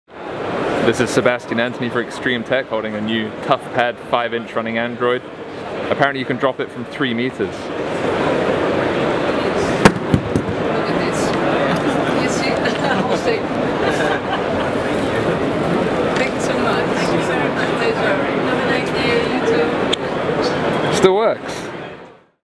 Panasonic Toughpad drop test
Watch as I drop the 5-inch Pansonic Toughpad FZ-E1, running a recent version of Android -- and it still works afterwards! (The flap on the right was already broken before the drop test, in case you were wondering. I guess someone dropped it from four meters before I got to it...)

It's also water proof, and you can smack it around pretty hard, without it sustaining any damage.